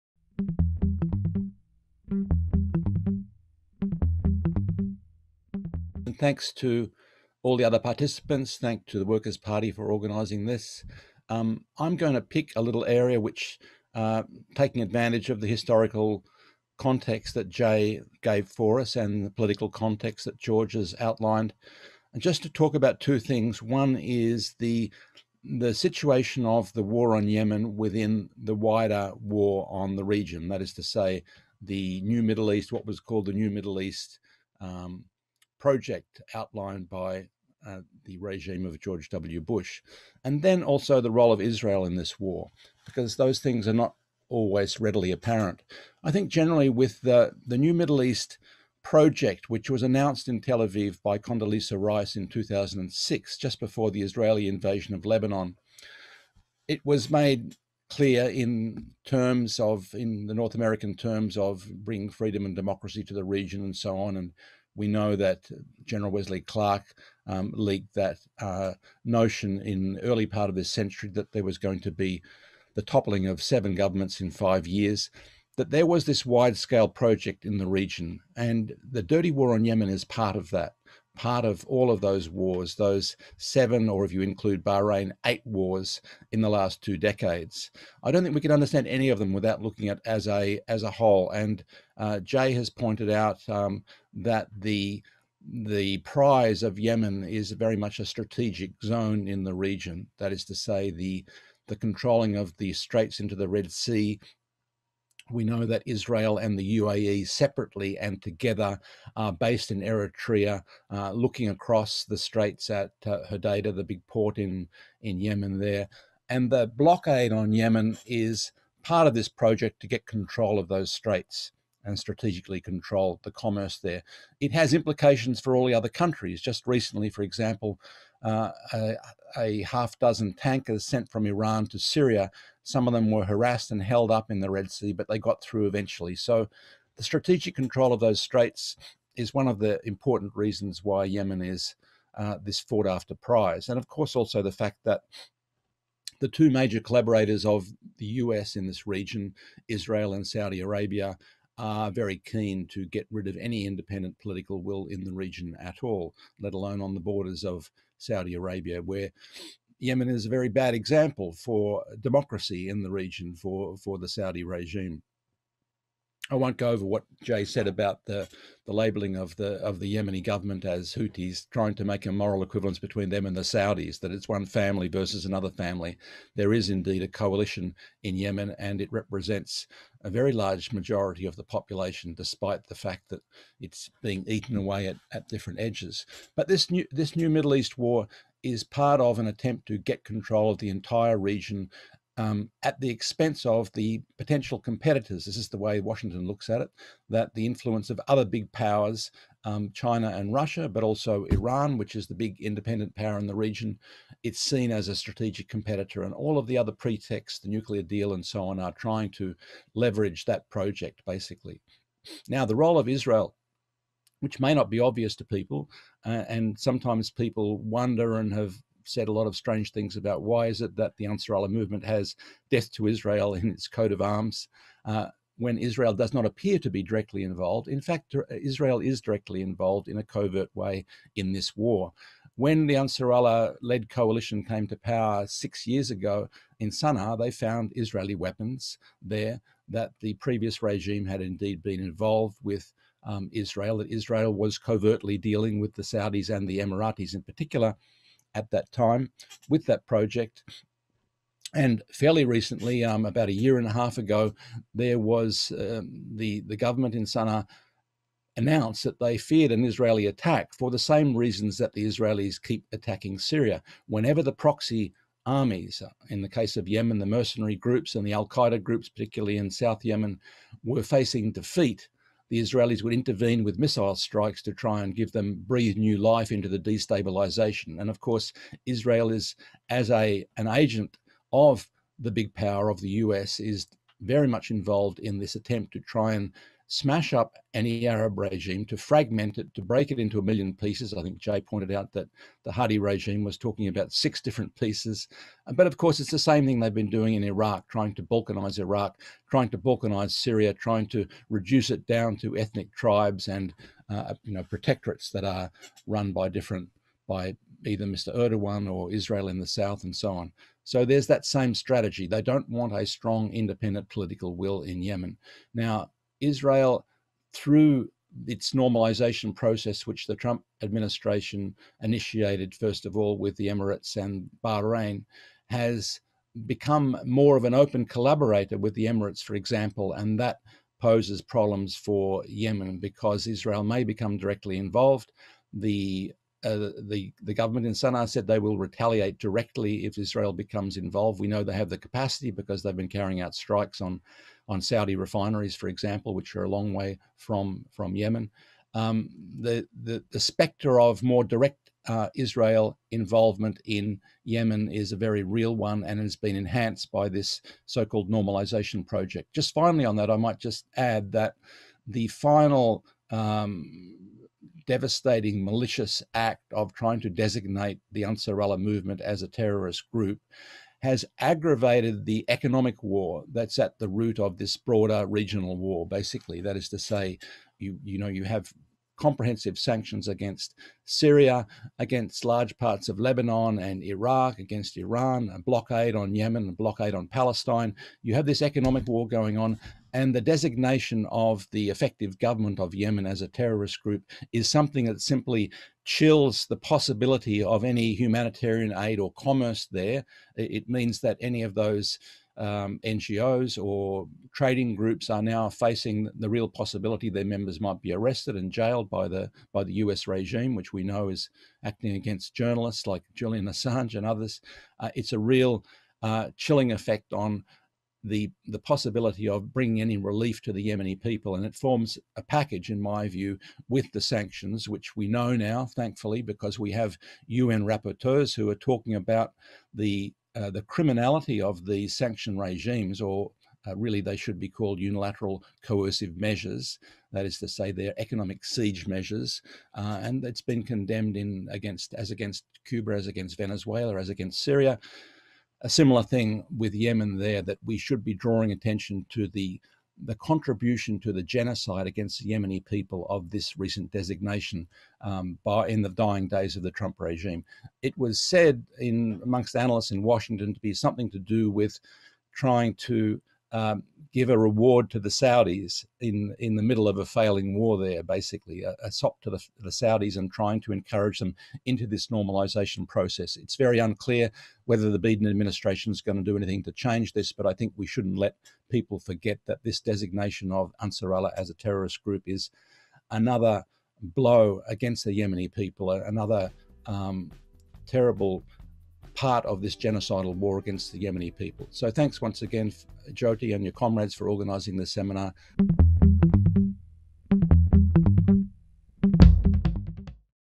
Tim Anderson: US and Israeli involvement in the war on Yemen
From the Great Britain Workers Party online seminar, originally on George Galloway's site. 27 January 2021.